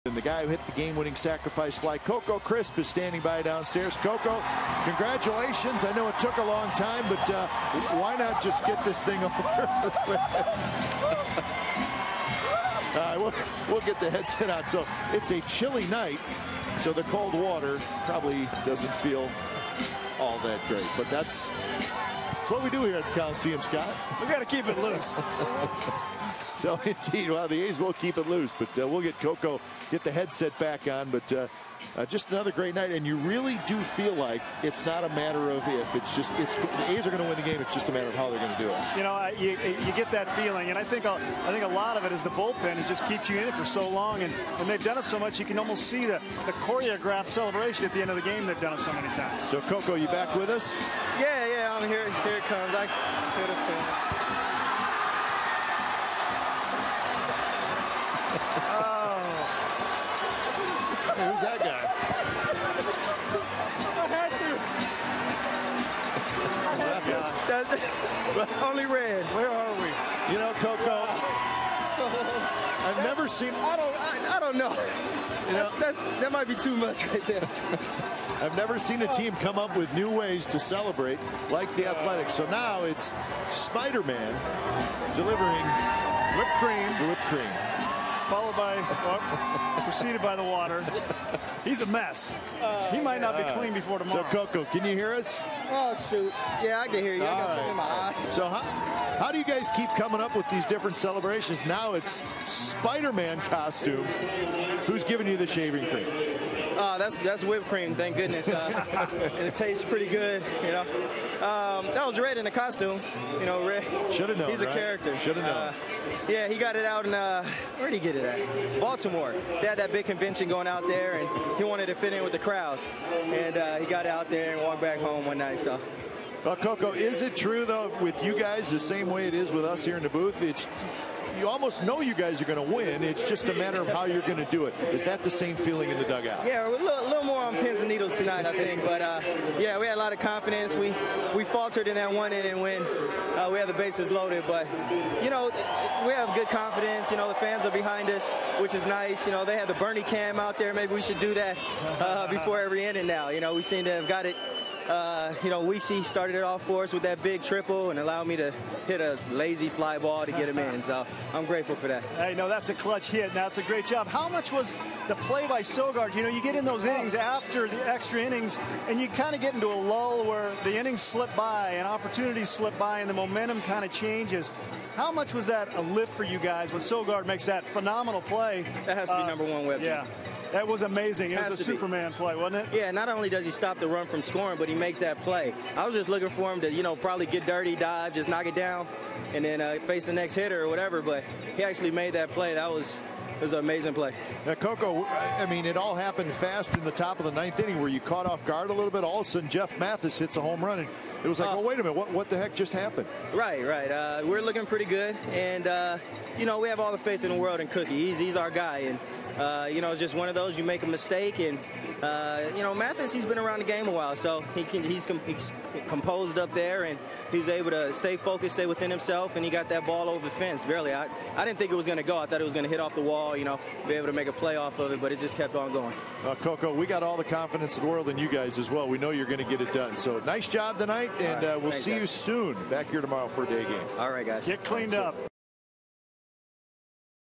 2012/08/03 Crisp on walk-off win
TOR@OAK: Crisp tries to talk about the walk-off win